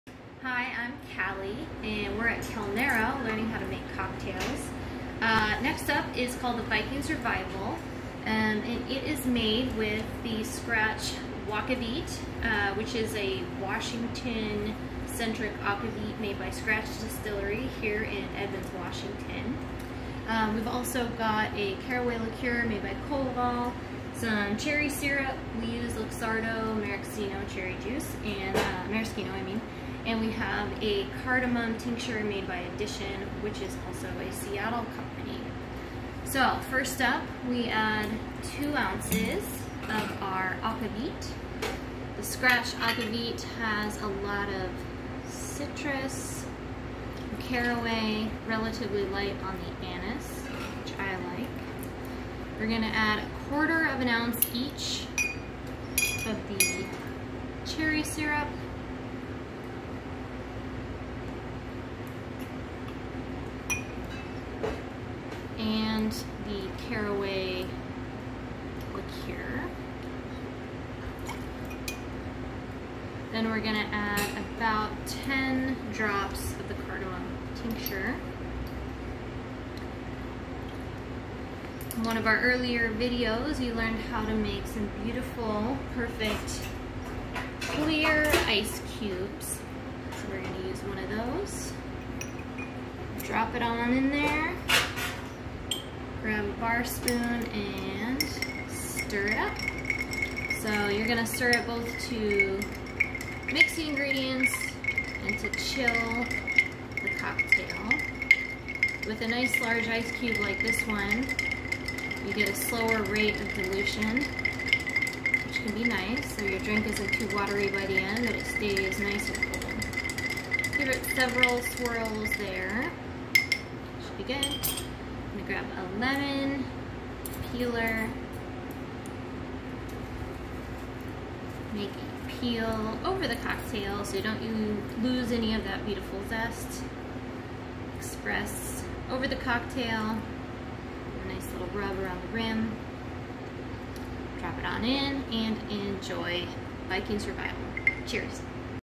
Viking's Revival
Learn how to make one of Kelnero's most popular cocktails from the comfort and safety of your own home! In this episode we'll make one of our signature cocktails, the Viking's Revival: 2 oz aquavit (we use Scratch Distillery's WAquavit), 0.25 oz cherry syrup (Luxardo), 0.25 oz caraway liqueur (Koval), and 10 drops of cardamom tincture (Addition). Stir in an old fashioned glass with a large, preferably clear, ice cube and garnish with an expressed lemon peel. Cheers!